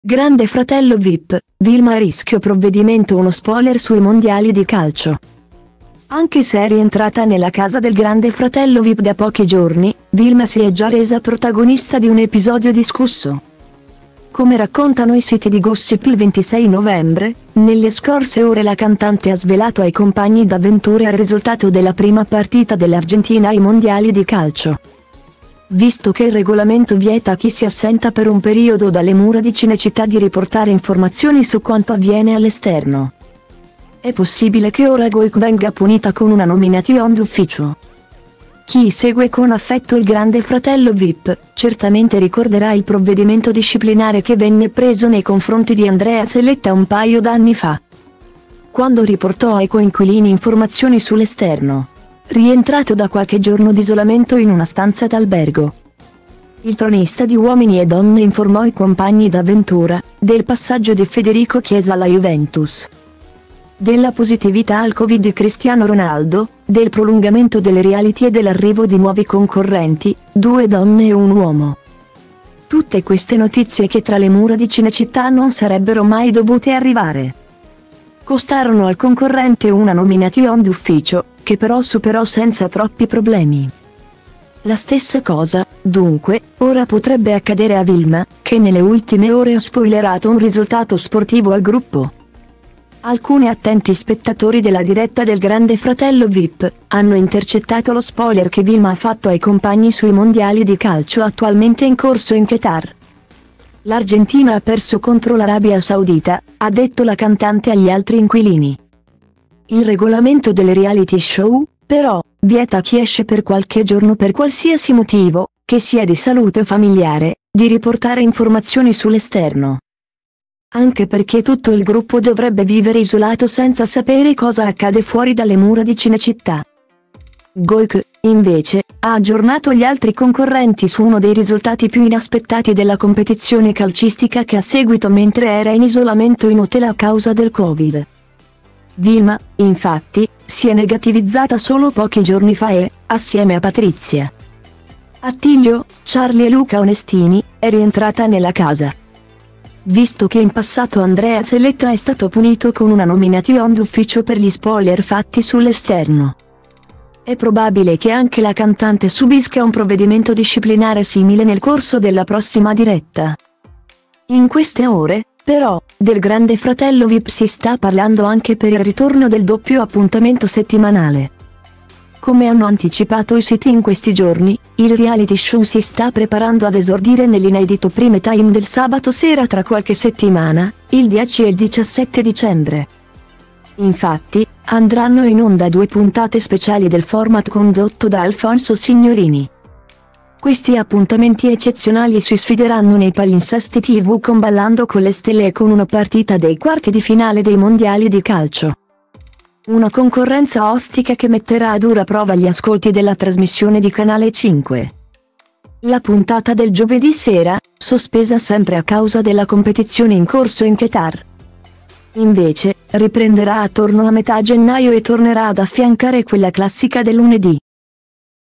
Grande Fratello Vip, Wilma a rischio provvedimento uno spoiler sui Mondiali di calcio
Anche se è rientrata nella casa del Grande Fratello Vip da pochi giorni, Wilma si è già resa protagonista di un episodio discusso. Come raccontano i siti di Gossip il 26 novembre, nelle scorse ore la cantante ha svelato ai compagni d'avventura il risultato della prima partita dell'Argentina ai Mondiali di Calcio.Visto che il regolamento vieta a chi si assenta per un periodo dalle mura di Cinecittà di riportare informazioni su quanto avviene all'esterno,